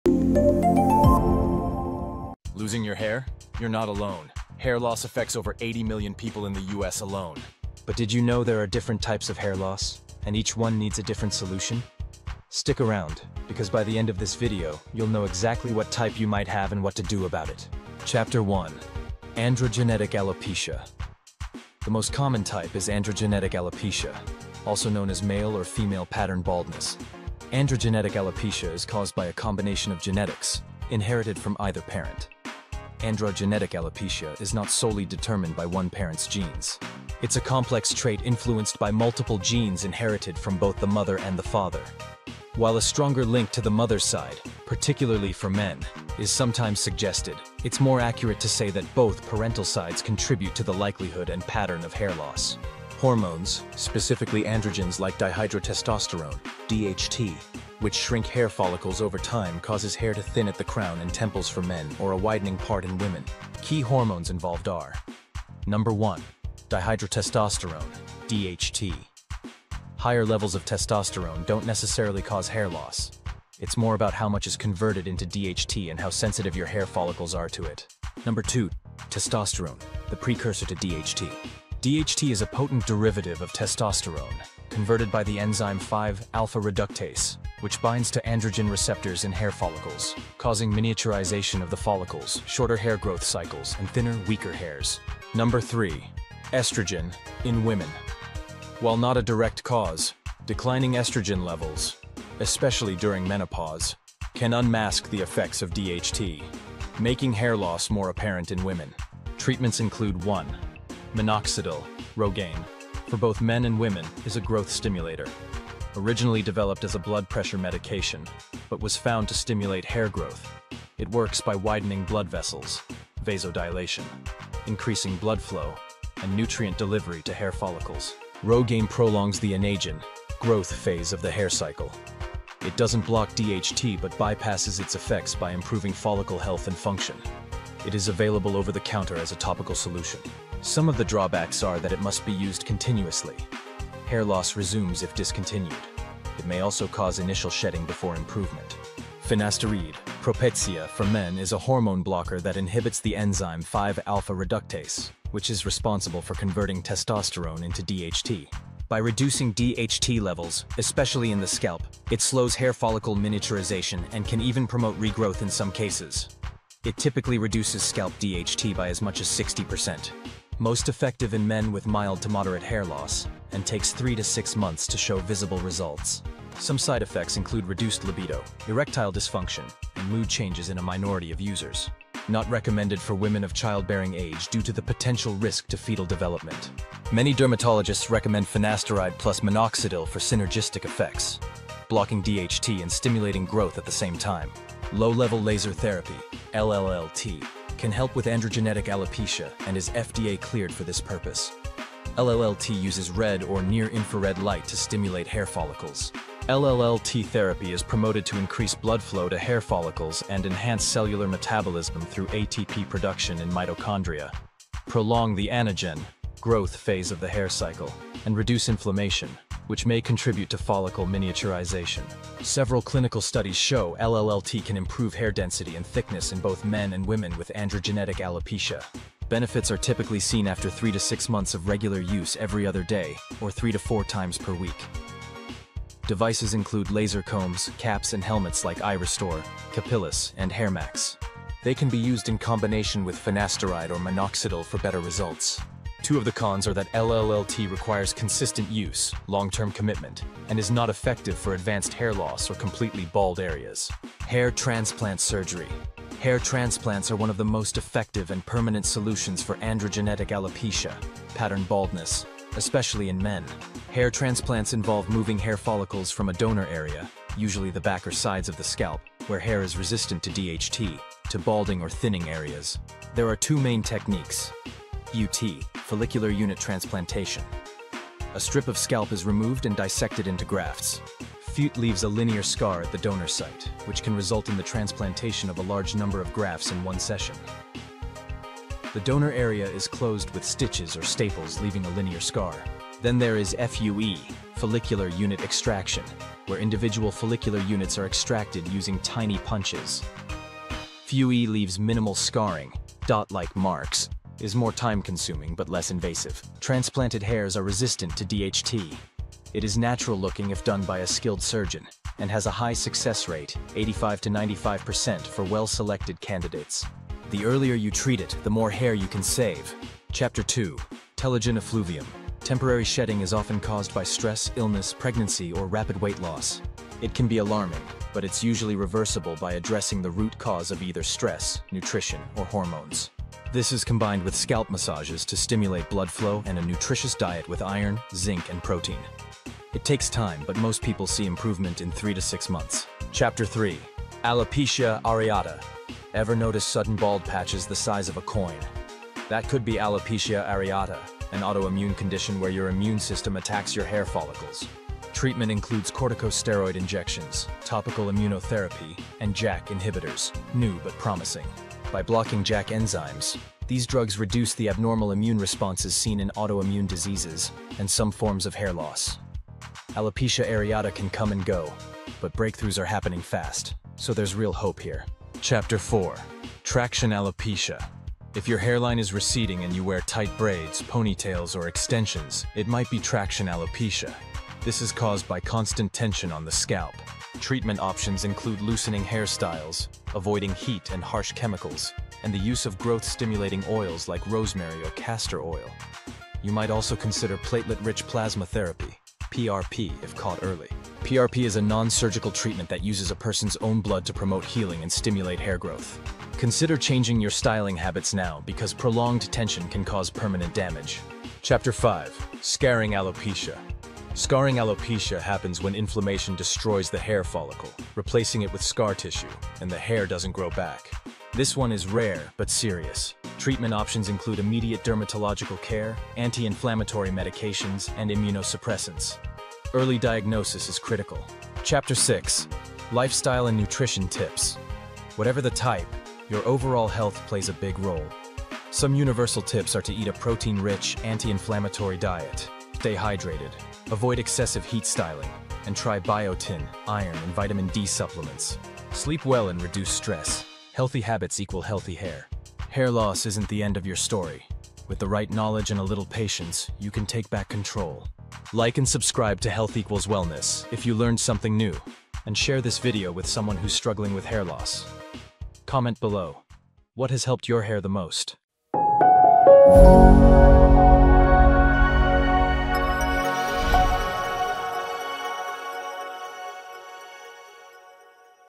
💇🏻‍♀️The Hidden Causes of Hair Loss No One Talks About👨🏼‍🦲
Struggling with hair loss? You're not alone. "Stop Hair Loss: Proven Tips That Work!" is your go-to in-depth guide to understanding the types of hair loss, critical warning signs, and the game-changing treatments available. From androgenetic alopecia and telogen effluvium to traction alopecia and scarring alopecia, we break down the causes, pioneering procedures, and medical marvels that are revolutionizing hair care. Learn about treatments like minoxidil, finasteride, low-level laser therapy, and hair transplants, plus lifestyle tips that can make all the difference.

This video highlights the global impact of hair loss while offering personalized, actionable solutions. Whether it’s addressing hormones like DHT, exploring the latest techniques like PRP and JAK inhibitors, or adopting healthy habits, we've got you covered. Your journey to healthier hair starts here.

CHAPTERS:
00:00 - Hair Loss Affects Over 80 Million People in the US
00:18 - Androgenetic Alopecia (Male or Female Pattern Baldness)
05:19 - Telogen Effluvium (Temporary Hair Shedding)
05:41 - Alopecia Areata (Sudden Bald Patches)
06:12 - Traction Alopecia (Receding Hairline from Tension)
06:49 - Scarring Alopecia (Permanent Hair Loss)
07:11 - Lifestyle and Nutrition Tips for Healthy Hair